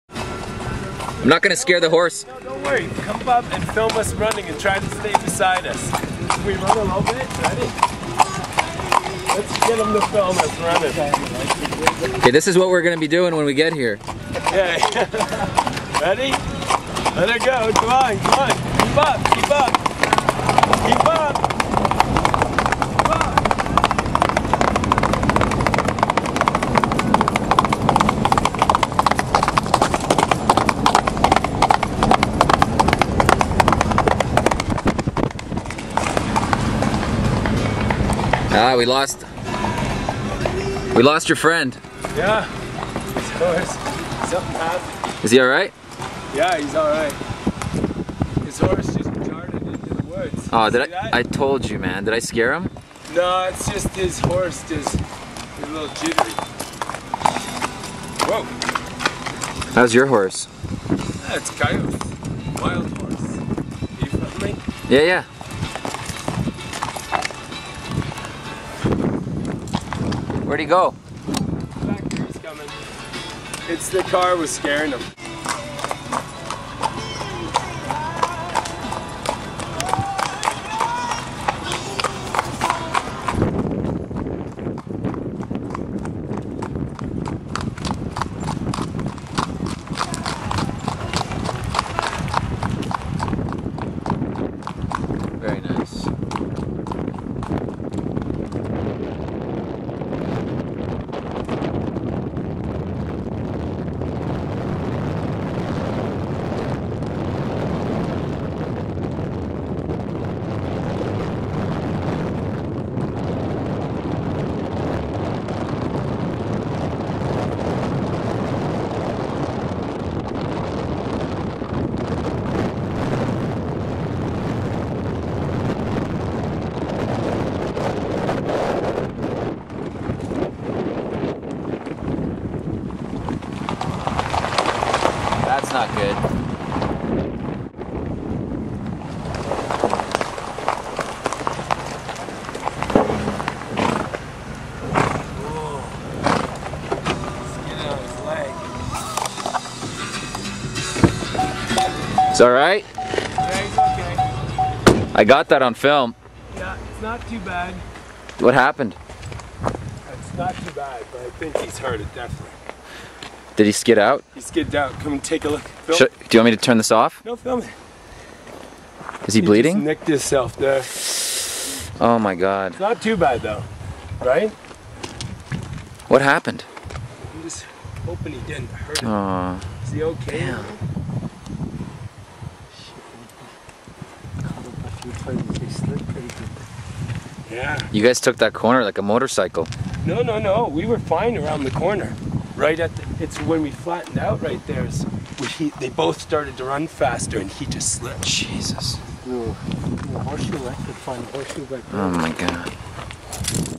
Drew Rouse in British Columbia wipes out on his Horse!!
The caped crusader takes a dive but gracefully recovers.  Unfortunatly the horse was sent to the glue factory hours later...   Just kidding